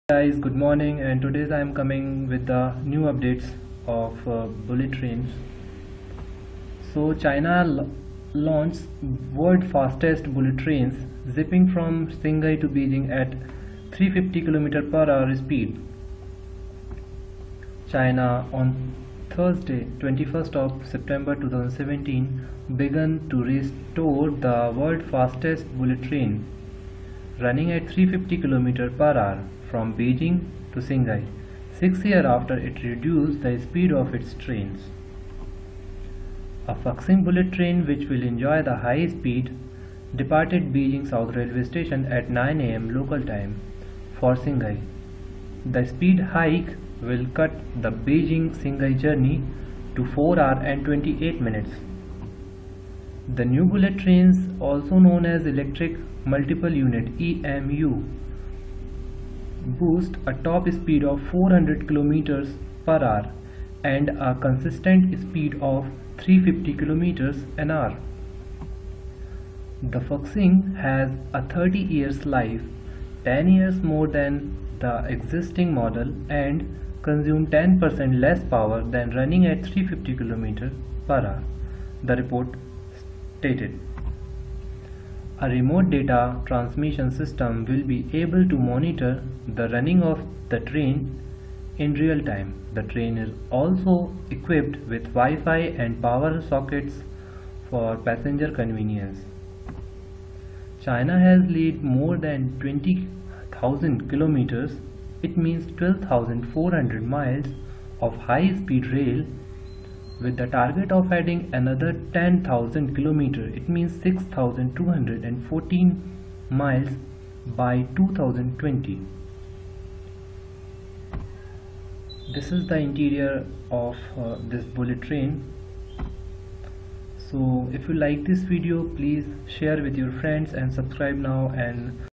520 kmph fast train in india
Today, seven pairs of the country's new homemade Fuxing bullet trains went into operation on the Beijing-Shanghai HSR, traveling at speeds of up to 350 km/h. Previously, trains in China could only crawl along at a mere 300 km/h, making the 1,318-kilometer trip between Shanghai and the capital in an excruciating 4 hours and 49 minutes. Thanks to the Fuxing speed boost, you can now make that trip in 4 hours and 28 minutes.